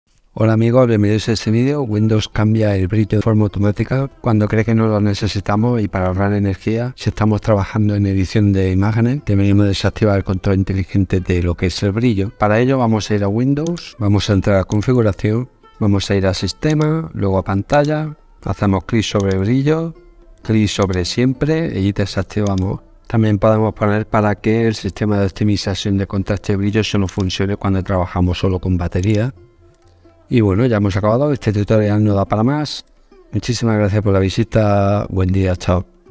Evitar cambios de brillo automáticos en Windows 11 ✅
👌¿Cansado de los cambios automáticos de brillo en Windows 11? Aprende a desactivarlos fácilmente,

Windows 11 cuenta con una función llamada control de brillo adaptativo de contenido, que ajusta automáticamente el brillo y contraste de la pantalla en función del contenido que se muestra, ahorrando energía y proporcionando una mayor duración de la batería. Sin embargo, si estos cambios automáticos te distraen o molestan, puedes desactivarlos siguiendo unos sencillos pasos en la configuración de pantalla. Desactivar esta función es fácil y te permitirá personalizar la configuración de brillo según tus necesidades y preferencias.